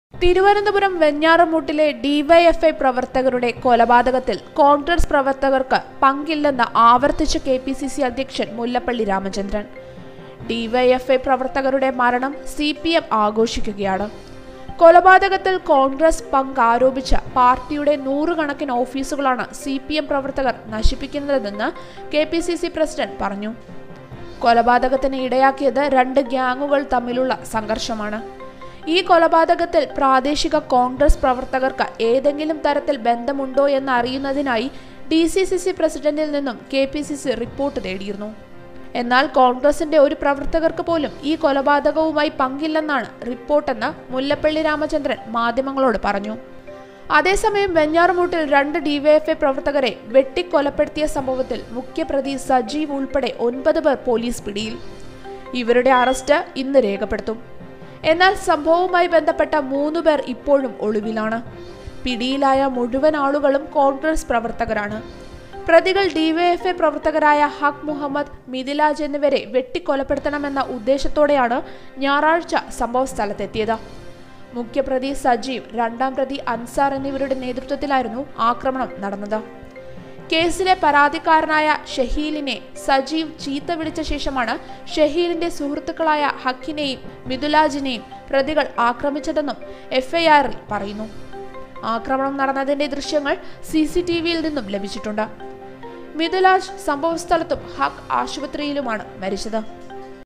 വെഞ്ഞാറമ്മൂട് ഇരട്ടക്കൊലപാതകം;കൊലപാതകത്തില്‍ കോണ്‍ഗ്രസിന് പങ്കില്ലെന്ന് മുല്ലപ്പള്ളി
Mullappally says Congress has no role in Venjarammoodu double murder